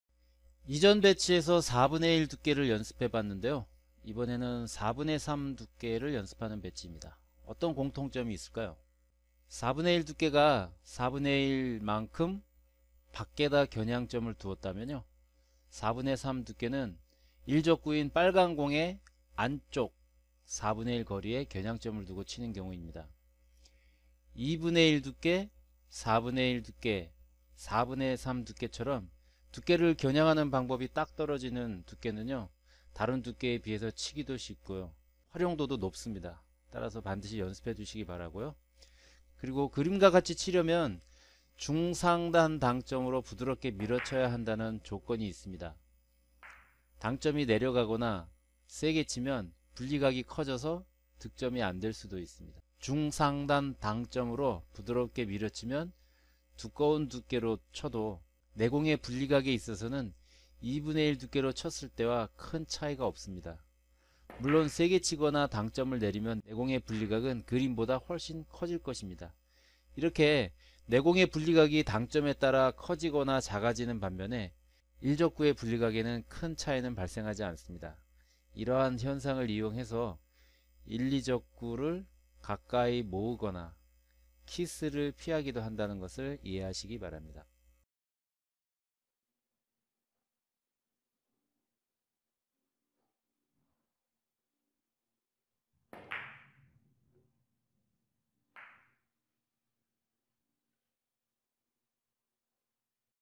4구 밀어치기 04 - 무회전 4분의 3 두께로 치기 (당구박사 4구 당구레슨)
* 무회전 3/4두께로 치는 연습입니다.